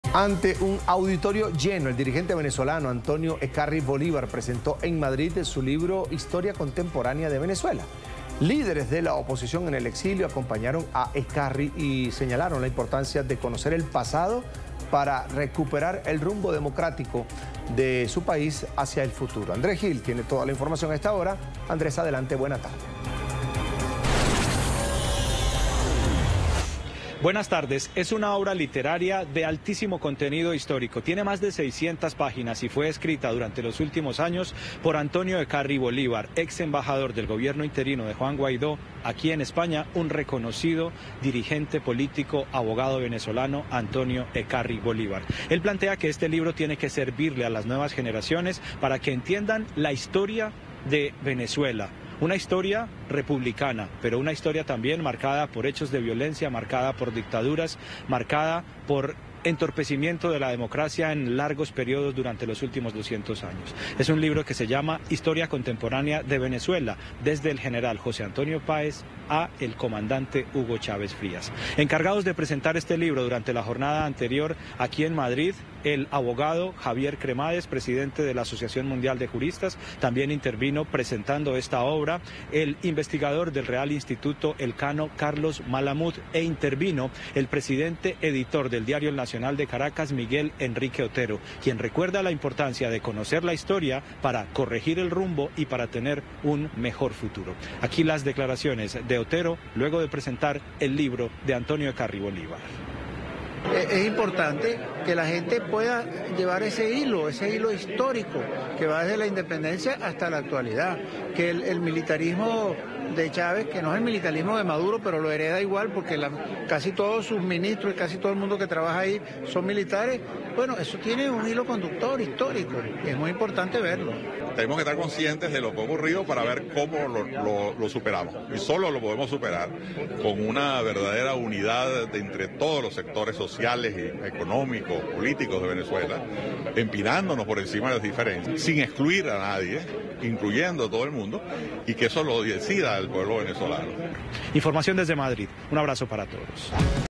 La historia de Venezuela desde el General José Antonio Páez a Hugo Chávez
Antonio Ecarri Bolívar dirigente venezolano y ex embajador del gobierno interino de Juan Guaidó, presentó en Madrid y ante un auditorio lleno su libro “Historia contemporánea de Venezuela”.

Líderes de la oposición en el exilio acompañaron a Ecarri y señalaron la importancia de conocer el pasado para recuperar el rumbo democrático de su país.